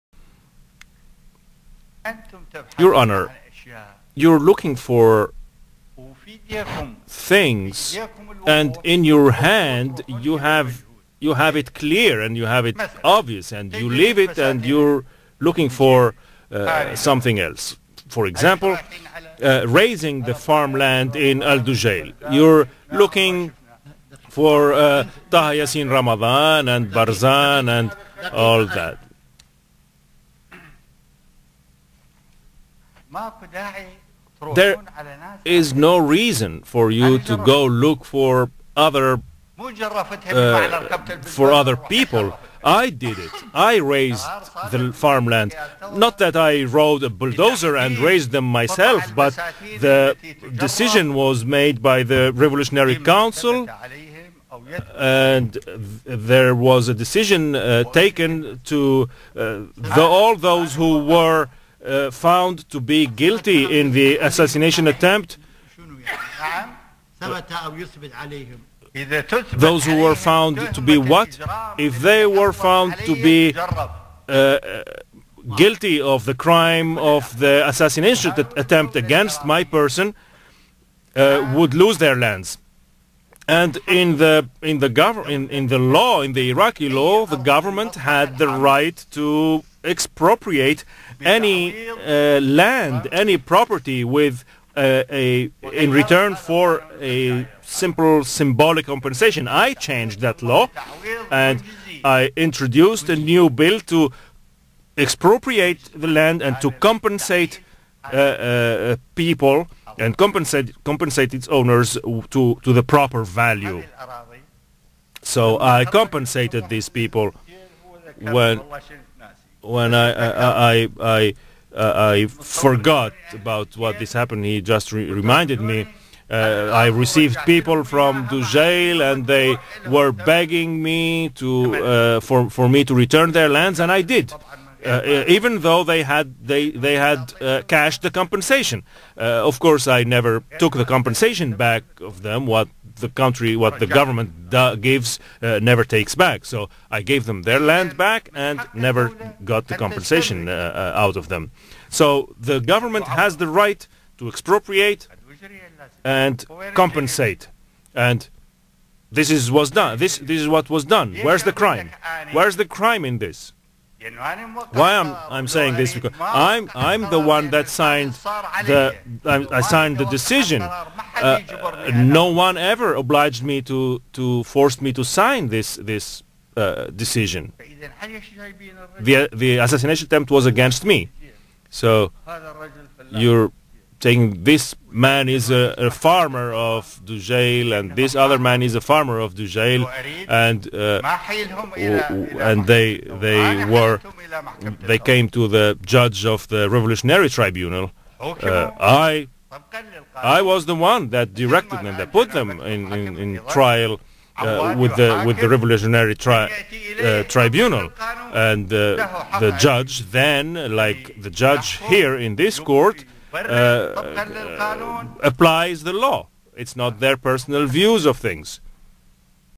Look Saddam confirms his responsibility for what happened and exonerates his comrades of accusations
Saddam witness confirms his responsibility for what happened and exonerates his comrades of the accusations.
شاهد صدام يؤكد مسؤوليته عن ماحدث ويبرئ رفاقه من الاتهامات.

Video description :
saddam says : and in your hand, you have you have it clear, and you have it obvious.
and you leave it and you're looking for something, else for example raising the farmland in jail you're looking for ramadan and barzan and all that for other people.

i did it i raised the farmland not that i rode a bulldozer and raised them myself but the decision was made by the revolutionary council and there was a decision taken to all those who were found to be guilty in the assassination attempt.

those who were found to be what if they were found to be guilty of the crime of the assassination attempt against my person would lose their lands and in the in the govern in the law in the iraqi law the government had the right to expropriate any land any property with a in return for a simple symbolic compensation i changed that law and i introduced a new bill to expropriate the land and to compensate people and compensate compensate its owners to to the proper value so i compensated these people well when i forgot about what this happened he just reminded me i received people from to jail and they were begging me to  for me to return their lands and i did even though they had they they had cash the compensation of course i never took the compensation back of them what the country what the government gives never takes back so i gave them their land back and never got the compensation out of them so the government has the right to expropriate and compensate and this is what's done this this is what was done where's the crime where's the crime in this why i'm i'm saying this because i'm i'm the one that signs i signed the decision no one ever obliged me to force me to sign this this decision the assassination attempt was against me so you're thinking this man is a farmer of the jail and this other man is a farmer of the jail and they they were they came to the judge of the revolutionary tribunal i i was the one that directed them they put them in trial with the with the revolutionary tribunal and the judge then like the judge here in this court applies the law it's not their personal views of things.

  " good greeting "
The official accounts on Instagram, Facebook, Twitter and Tiktok, Teespring. for Iraq Archives - Archives Iraq.